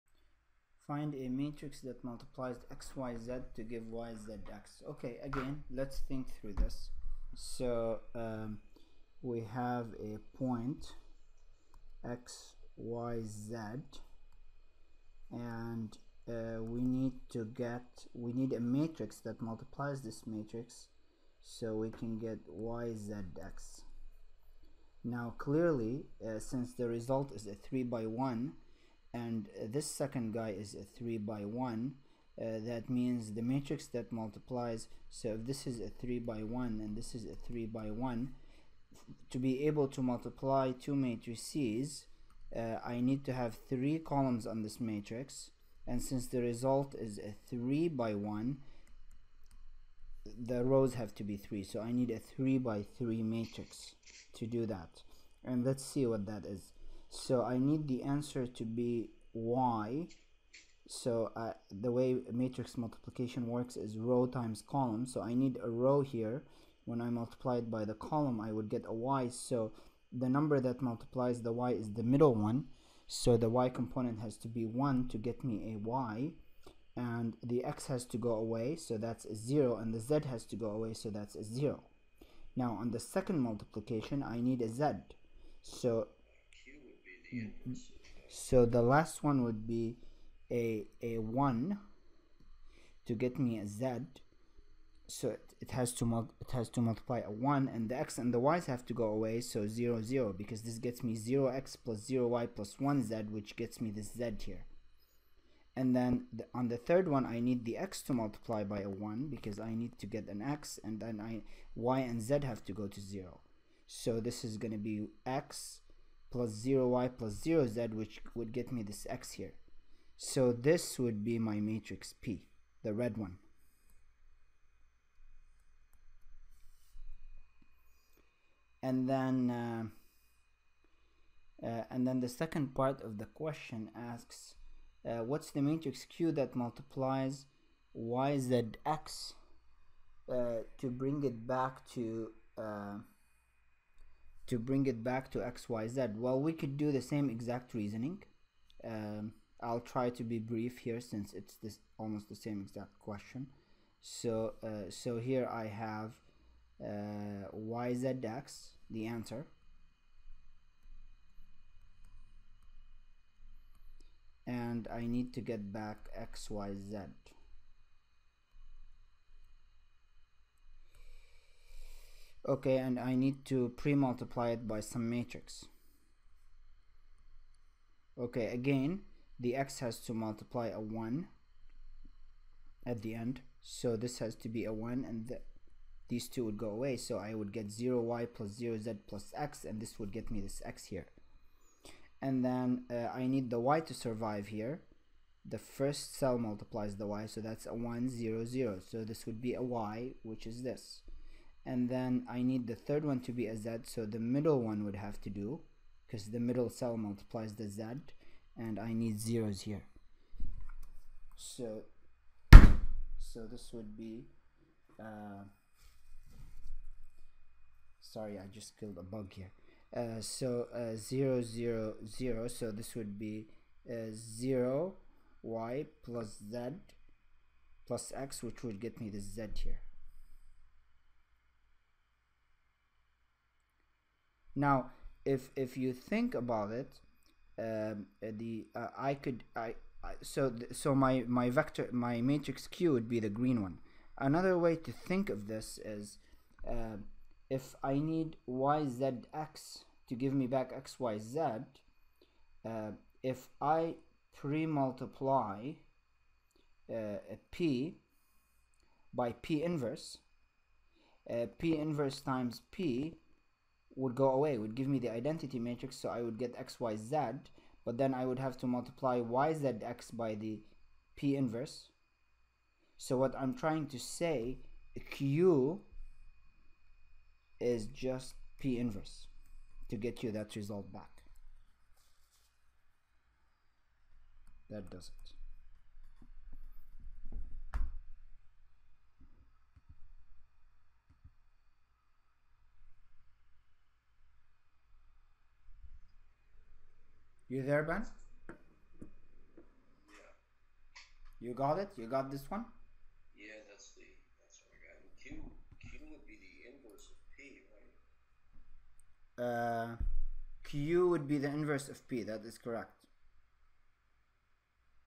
Find matrix P that changes x, y, z to y, z, x matrix Q that changes (y, z, x) to (x, y, z) 2-1-17
Introduction to Linear Algebra
Strang 4th edition
2-1-17
Find the matrix P that multiplies (x, y, z) to give (y, z, x). Find the matrix Q that
multiplies (y, z, x) to bring back (x, y, z).
matrix multiplication